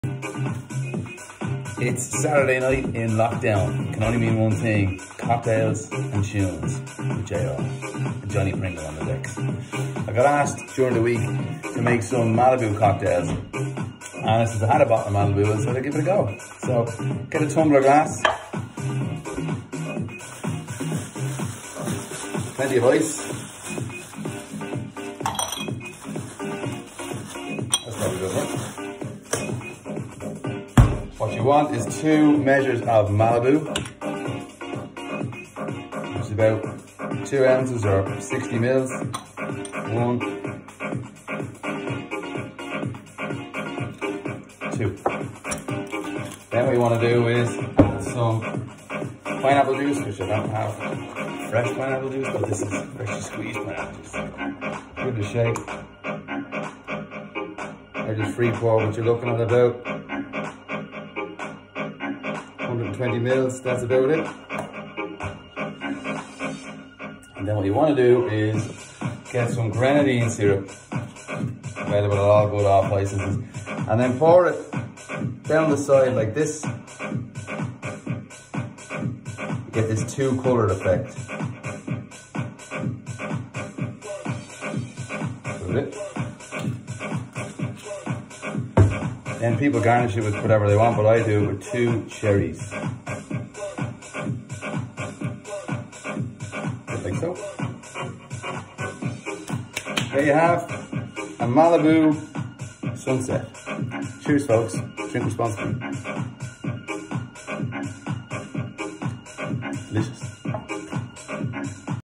Cocktails & Choons 5 - The Malibu Sunset 🍹🌅
Another installment in JR’s Cocktails & Choons series.

This week is probably a bit premature of a summary cocktail but sure it’s summer somewhere!🌅☺️. Another personal favourite of mine - what’s not to love?🍹🎉🕺🏻💃🏻

Follow JR @jayorr316

The tunes as always supplied by Jonny Pringle and are available at the below Soundcloud link.

JR.